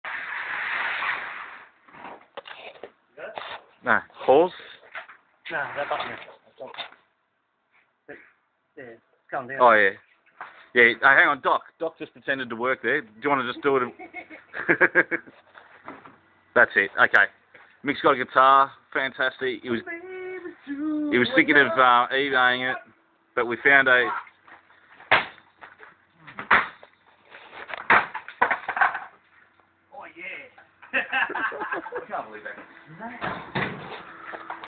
Smashing my guitar...
Smashing my guitar. Always wanted to do it. Now i have.